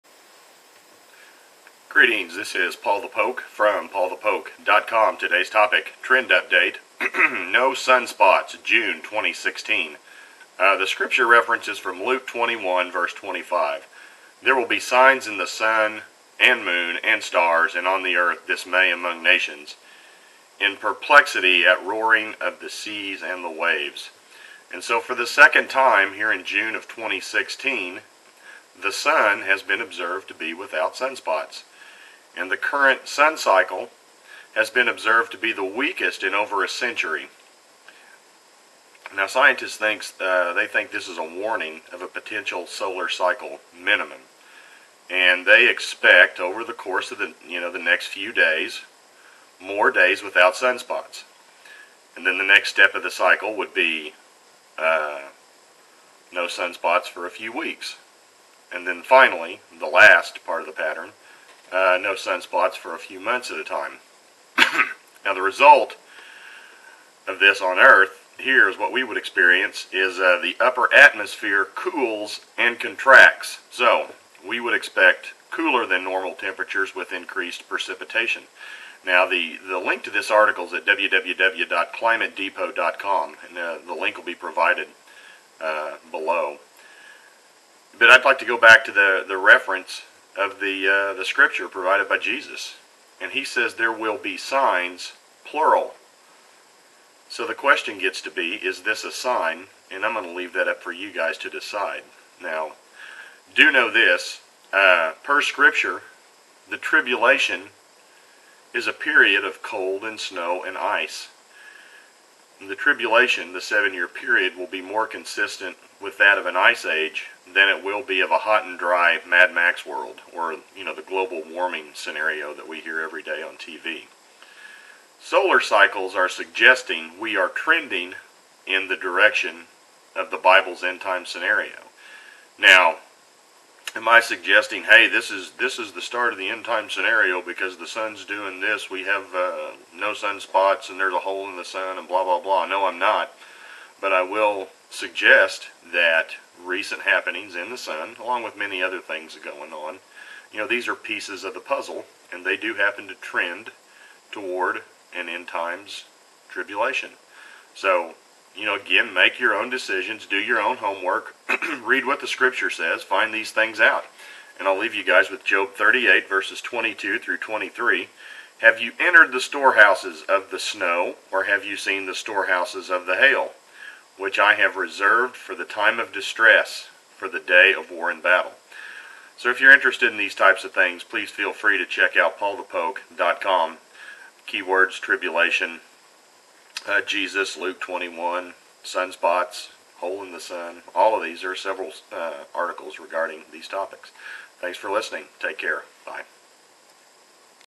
Trend Update: No Sun Spots, June 2016
NASA reports there are no sun spots on the sun for the second time in June 2016. Is this a sign? Investigate...